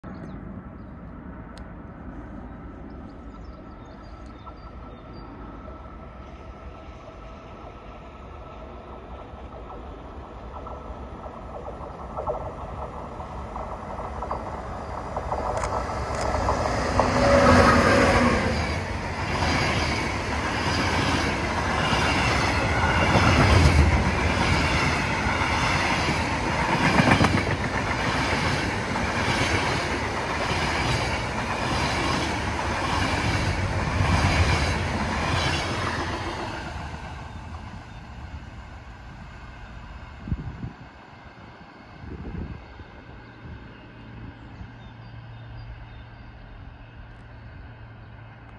Der ÖBB Taurus 1016 041 mit einem NightJet
Der ÖBB Taurus 1016 041 mit einem NightMJet. Auch über eure Kommentare würde ich mich freuen. Viel Spaß beim zuschauen euer Tammy Company 🚂🚃🚃🚃🚃

und vielleicht auch das Video teilen